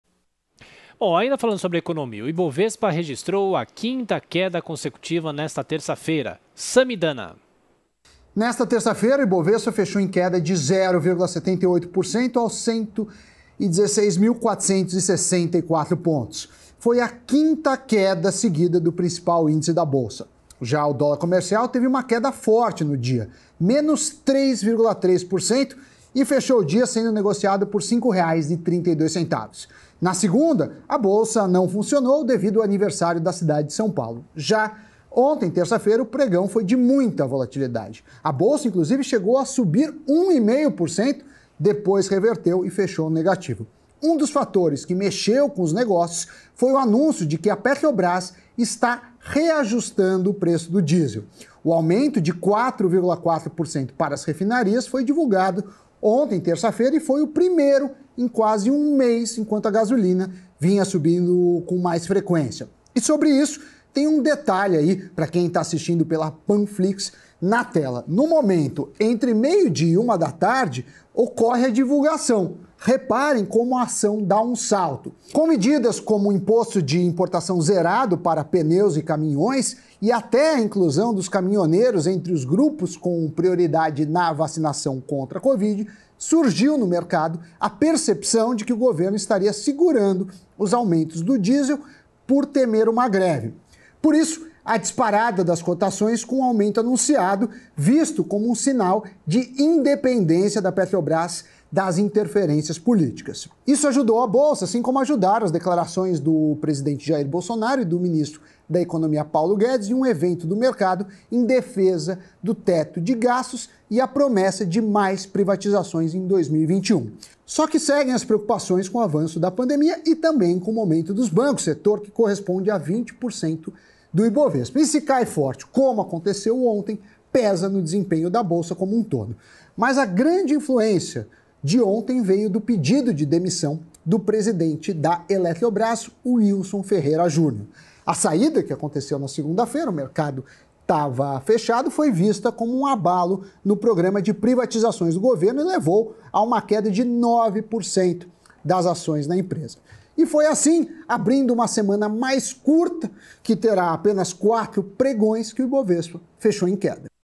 Samy Dana: Ibovespa registra quinta queda consecutiva
Principal índice da bolsa, Ibovespa, tem quinta queda seguida nesta terça-feira (26). Um dos fatores que mexeu com os negócios foi o anúncio de que a Petrobas está reajustando o preço do diesel. Samy Dana traz os detalhes.